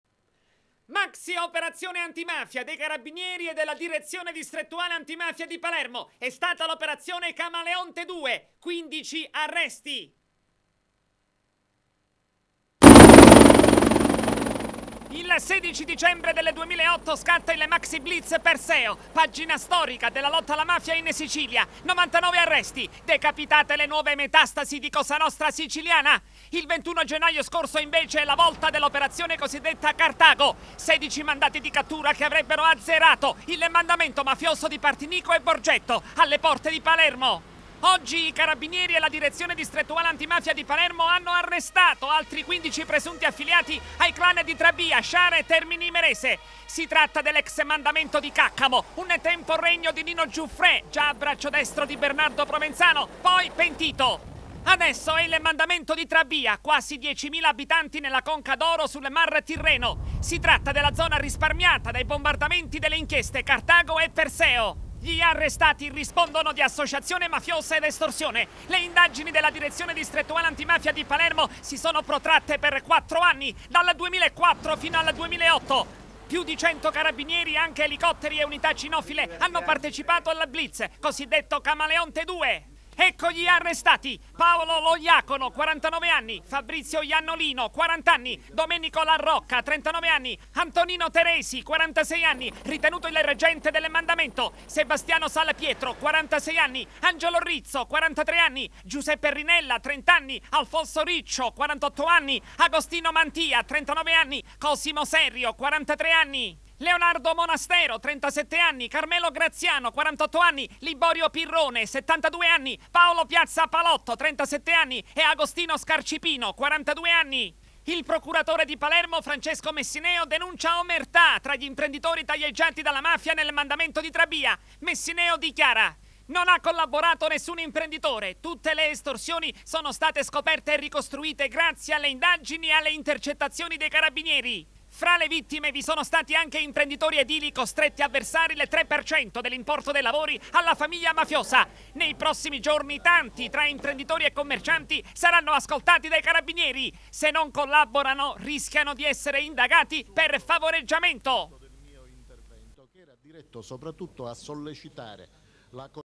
Ruoppolo Teleacras - Operazione "Camaleonte 2"
Il servizio di Angelo Ruoppolo Teleacras Agrigento ) del 17 marzo 2009. Maxi blitz antimafia dei Carabinieri e della Direzione distrettuale antimafia di Palermo. Decimati i clan di Trabia, Sciara e Termini Imerese. 15 arresti (per il video seleziona Alta qualità cliccando su HQ in basso a destra dello schermo).
Il 16 dicembre del 2008 scatta il maxi blitz  "Perseo", pagina storica della lotta alla mafia in Sicilia. 99 arresti. Decapitate le nuove metastasi di Cosa nostra siciliana. Il 21 gennaio scorso, invece, è la volta della operazione cosiddetta "Carthago". 16 mandati di cattura che avrebbero azzerato il mandamento mafioso di Partinico e Borgetto, alle porte di Palermo. Si tratta dell'ex mandamento di Caccamo, un tempo regno di Nino Giuffrè, già braccio destro di Bernardo Provenzano, poi pentito. Adesso è il mandamento di Trabia, quasi 10mila abitanti nella Conca d'Oro, sul mar Tirreno. Si tratta della zona risparmiata dai bombardamenti delle inchieste "Carthago" e "Perseo". Gli arrestati rispondono di associazione mafiosa ed estorsione. Le indagini della Direzione distrettuale antimafia di Palermo si sono protratte per 4 anni, dal 2004 fino al 2008. Più di cento Carabinieri, anche elicotteri ed unità cinofile,  hanno partecipato al blitz, cosiddetto "Camaleonte 2" . Ecco gli arrestati: Paolo Lo Iacono, 49 anni.  Fabrizio Iannolino, 40 anni. Domenico La Rocca, 39 anni.  Antonino Teresi, 46 anni, ritenuto il reggente del mandamento.  Sebastiano Salpietro, 46 anni. Angelo Rizzo, 43 anni. Giuseppe Rinella, 30 anni. Alfonso Riccio, 48 anni. Agostino Mantia, 39 anni. Cosimo Serio, 43 anni. Leonardo Monastero, 37 anni. Carmelo Graziano, 48 anni. Liborio Pirrone, 72 anni. Paolo Piazza Palotto, 37 anni. Agostino Scarcipino, 42 anni. Il procuratore di Palermo, Francesco Messineo, denuncia  omertà tra gli imprenditori taglieggiati dalla mafia nel mandamento di Trabia. Messineo dichiara :  "non ha collaborato nessun imprenditore. Tutte le estorsioni sono state scoperte e ricostruite grazie alle indagini ed alle intercettazioni dei Carabinieri. Fra le vittime vi sono stati anche imprenditori edili costretti a versare il tre per cento dell'importo dei lavori alla famiglia mafiosa. Nei prossimi giorni tanti, tra imprenditori e commercianti, saranno ascoltati dai Carabinieri. Se non collaborano rischiano di essere indagati per favoreggiamento" .